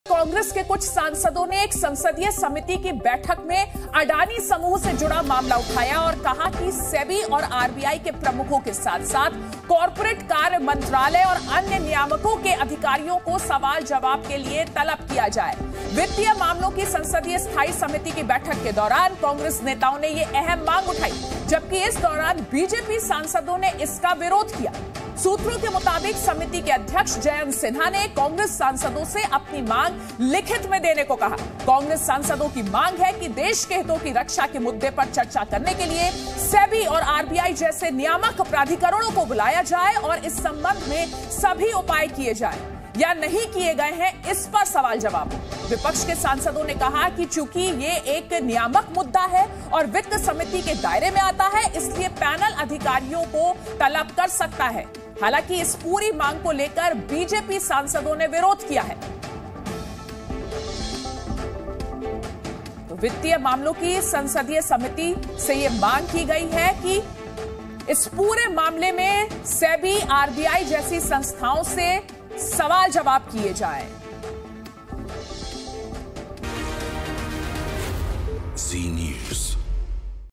Adani Case: वित्तीय मामलों की संसदीय समिति से Congress सांसदों की मांग, SEBI प्रमुख को करें तलब
कांग्रेस के कुछ सांसदों ने संसदीय बैठक में अडानी से जुड़ा मामला उठाया और वित्तीय मामलों की संसदीय समिति से बड़ी मांग रखी। कांग्रेस सांसदों ने कहा SEBI, RBI प्रमुखों के समेत कॉर्पोरेट कार्य मंत्रालय और अन्य नियामकों को सवाल जवाब के लिए बुलाया जाए। इस मांग का बीजेपी सांसदों ने जमकर विरोध किया।

Some Congress MPs raised the matter related to Adani in parliamentary meeting and placed a big demand before the parliamentary committee of financial affairs. Congress MPs said SEBI, Ministry of Corporate Affairs and other regulators including RBI chiefs should be called for questioning. BJP MPs strongly opposed this demand.